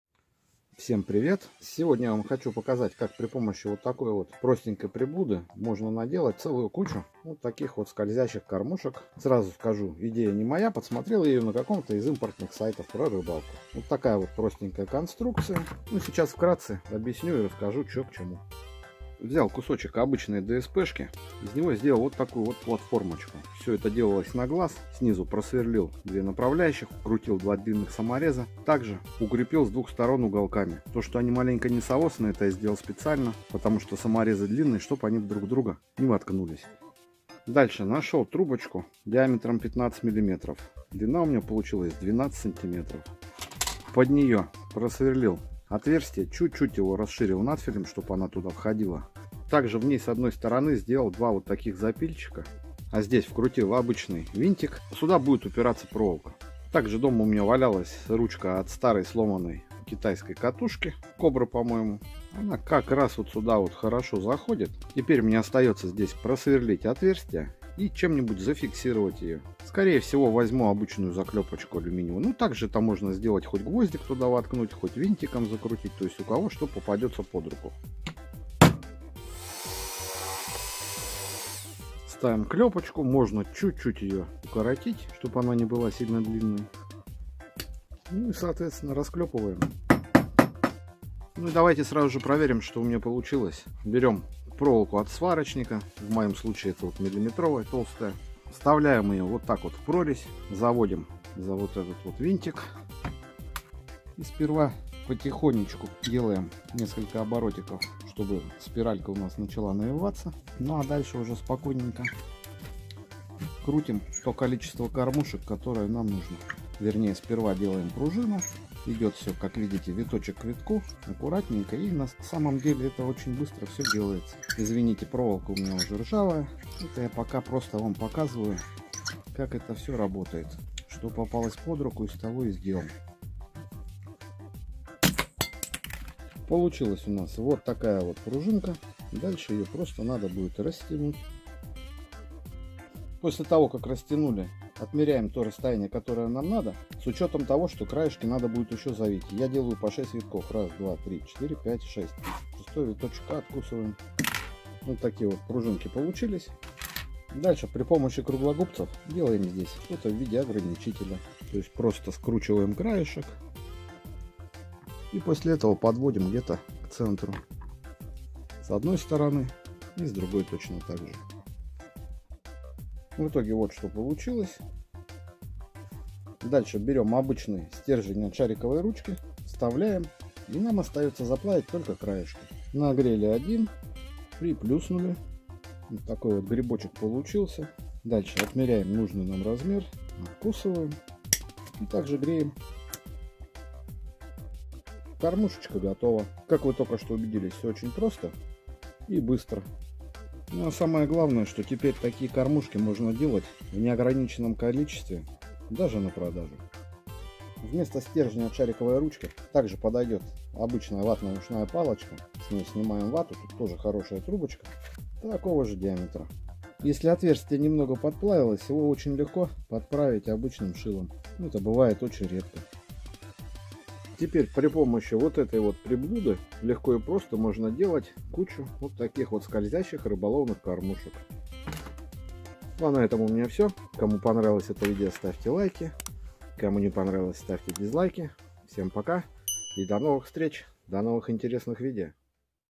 С помощью этого устройства вы легко наделаете целую кучу кормушек-пружин для рыбалки!
Сделав подобную приспособу вы сможете делать кормушки-пружины в неограниченном количестве!!!
Необходимые товары для рыбалки я заказываю на Яндекс Маркете. Можете убедиться сами пройдя по ссылке приведённой ниже, возможно и вас что-то заинтересует